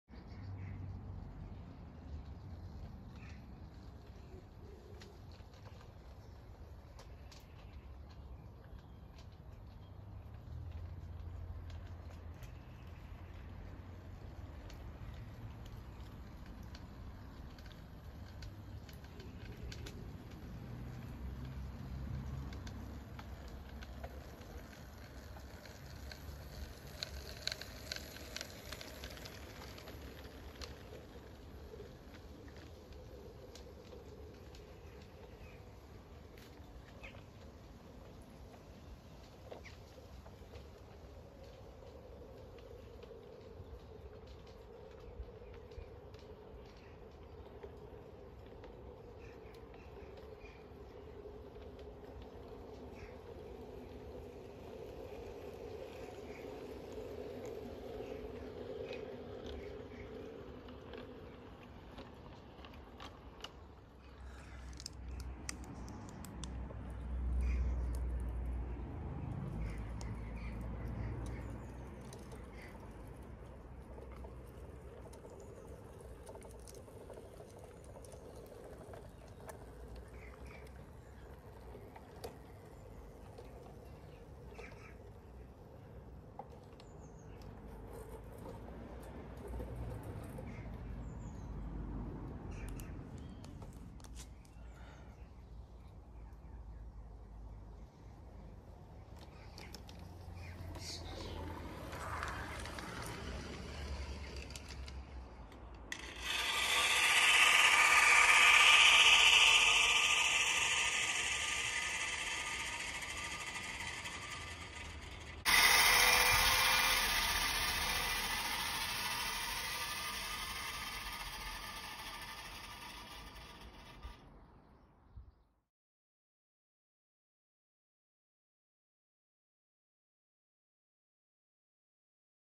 FeckHallBahn Morning Journeys 20th June
Quarry Hunslet Alice class Number 10 with the milk train followed by Stainz 0-4-0T Number Two with the early morning farm freight, finishing up with the first ever commuter journey bringing passengers from Schloß Alten Rasenmäher to Hornundtrompetebahnhof.